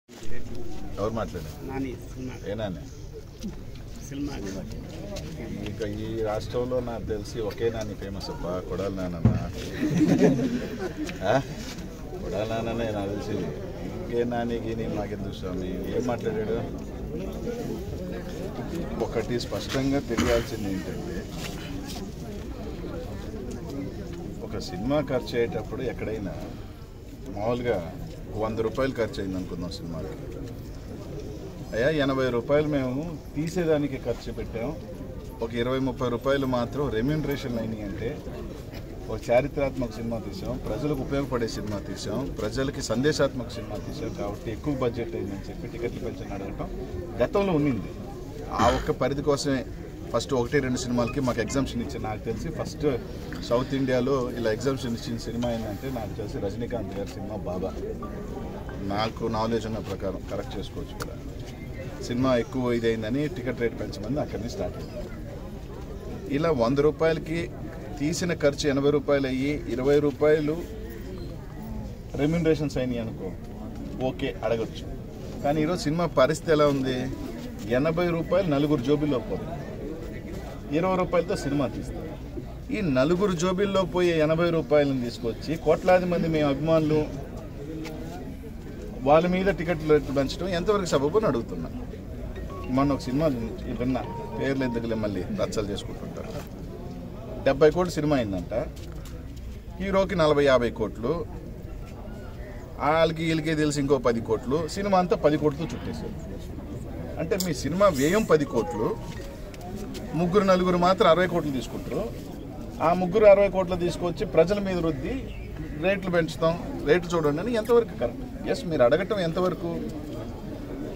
Anil Kumar Yadav Strong Counter To Hero Nani About His Words On Tickets | Sahithi Tv
Watch here Anil Kumar Yadav Strong Counter To Hero Nani About His Words On Tickets